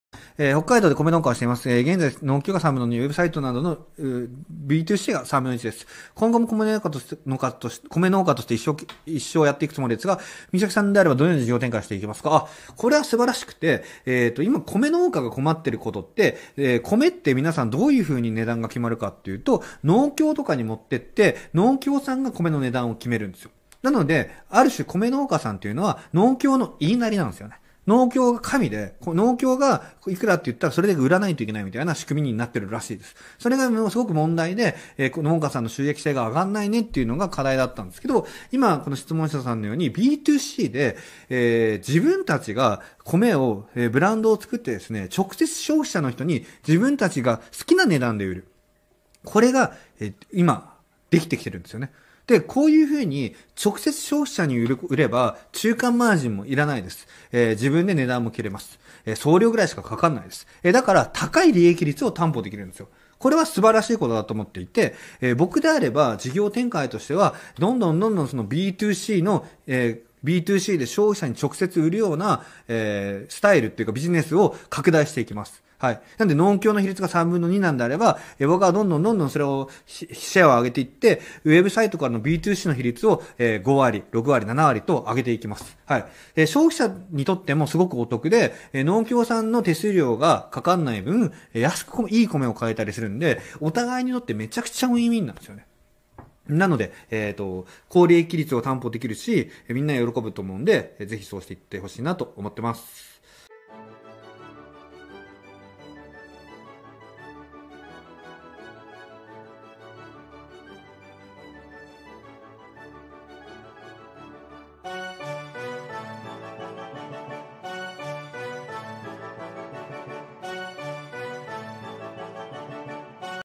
【青汁切り抜き】知ってる？BtoCビジネス今めっちゃ流行ってるよ？
ある質問からでた最近流行りのビジネスモデル？
知らない人が損する話を青汁王子の考えと共に語ってくれます。
しっかり学びましょう！

青汁王子の考え方、行動力を
切り抜きという形で力を貸し
発信して行きたいと考えてます。
意見、感想のコメントお待ちしてます
※当チャンネルは、ガジェット通信クリエーターネットワーク事務局と契約して運営しています

■その他